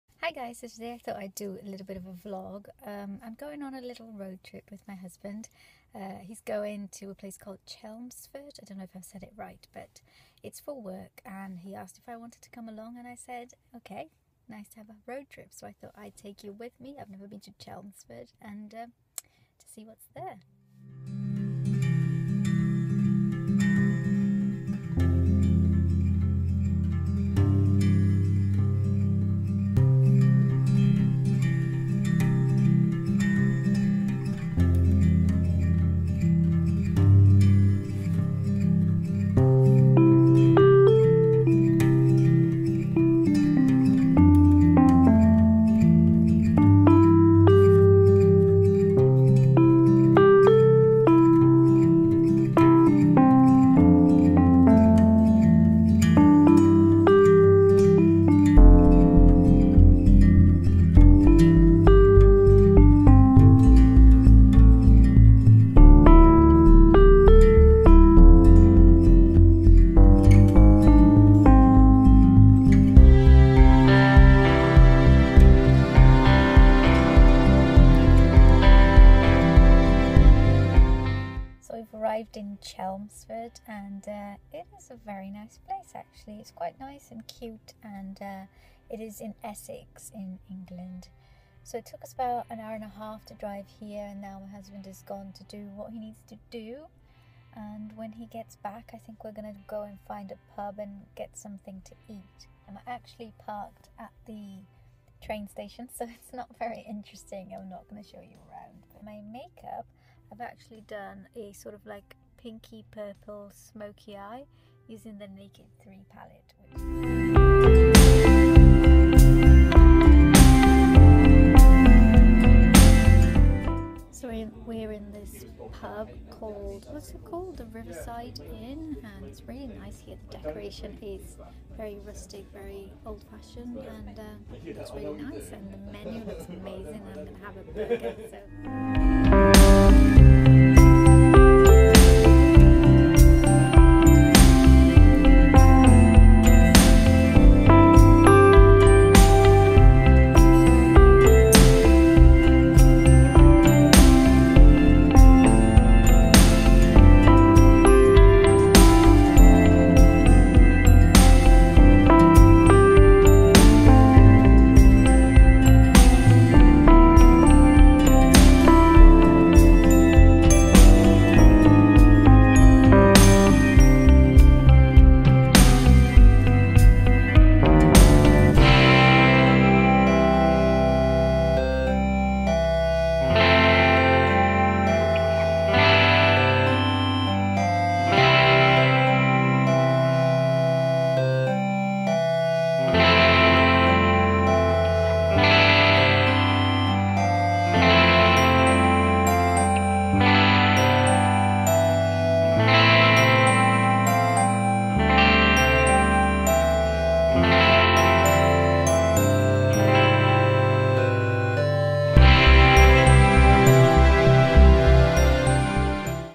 Vlog: Day Trip to Chelmsford! | Le Beauty Girl
My beauty & fashion blog

A short & sweet vlog from our day trip to Chelmsford! I hope you like it :) It was extremely hard persuading my husband to let me show him in this vlog, even if was just the side of his face or his shoes! :)

Chanel Vitalumiere Aqua Foundation
Mac Pro Longwear Concealer
Mac Pinch Me Blush
Soap & Glory Super Colour Fabulipstick in The Missing Pink
Rimmel 60 Seconds Nail Varnish in Caramel Cupcake

Primark Coat
Primark Vest Top
Primark Cardigan
Topshop Leigh Jeans
Dune Boots